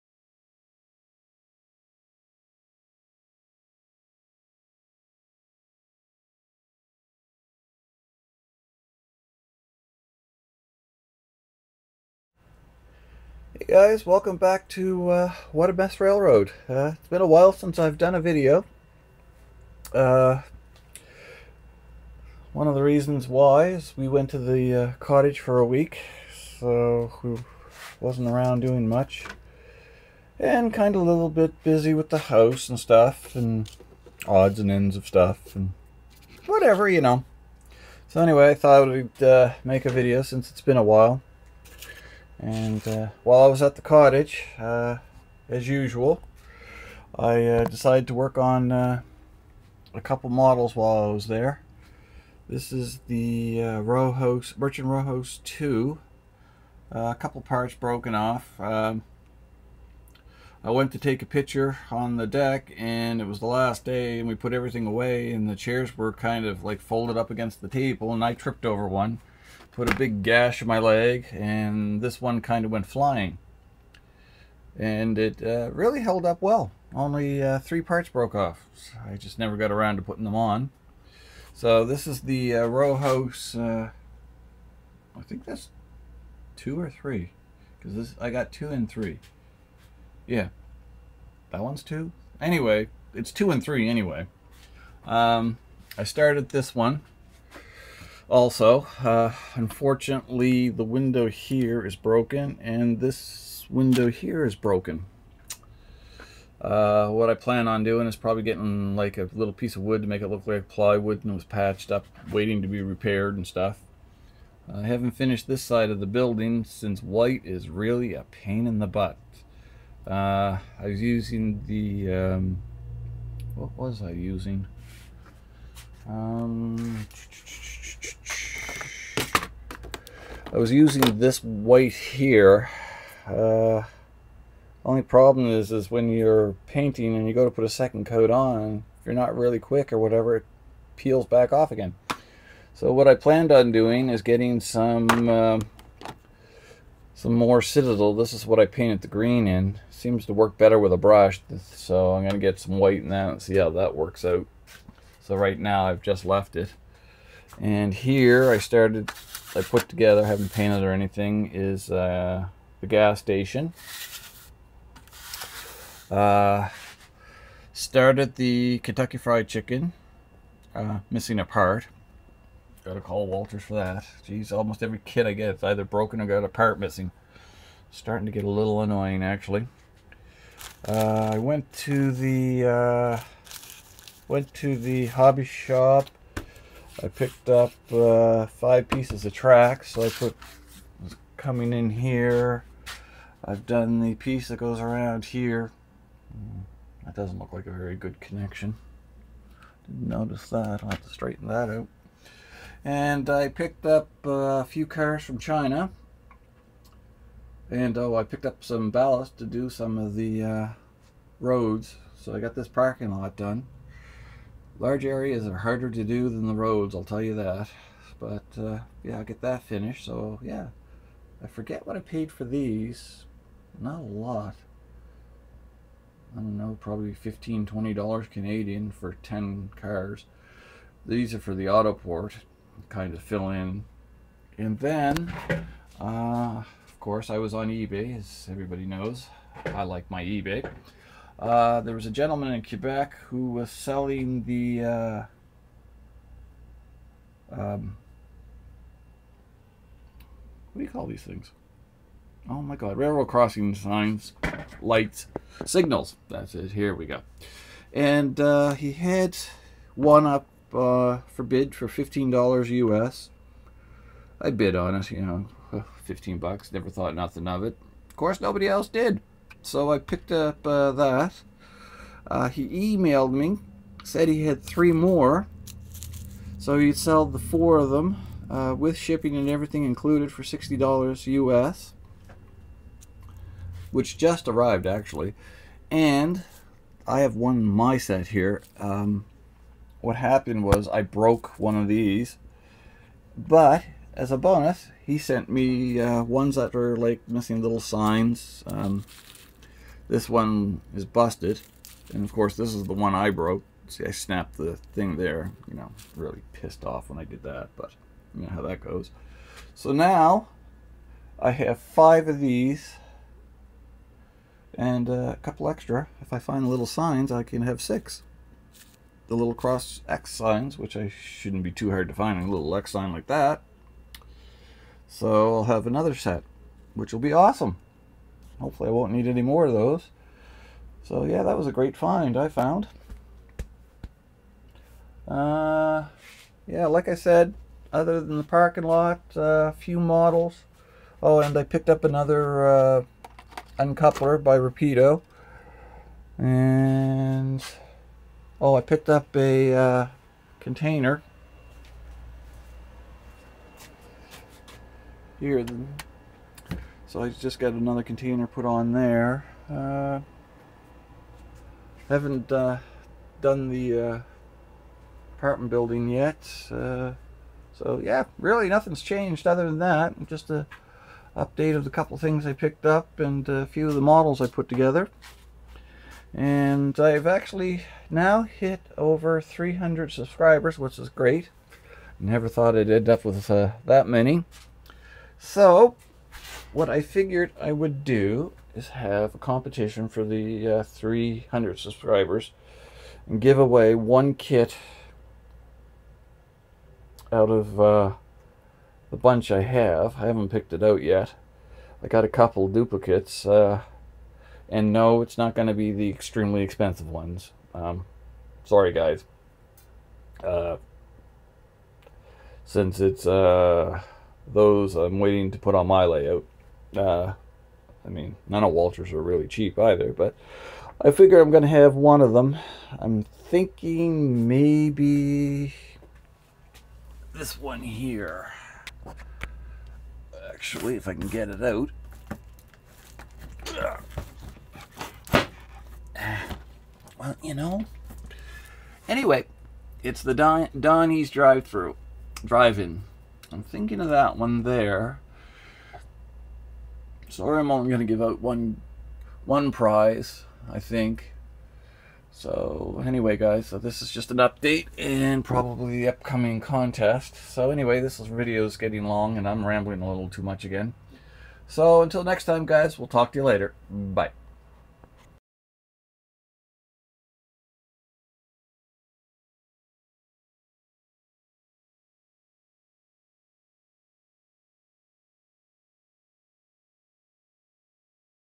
Back with a update.
Have been busy at the cottage and working on the house along with other things.  Did get a few things done on the layout. Picked up a few things for the layout. Will be having an up coming contest.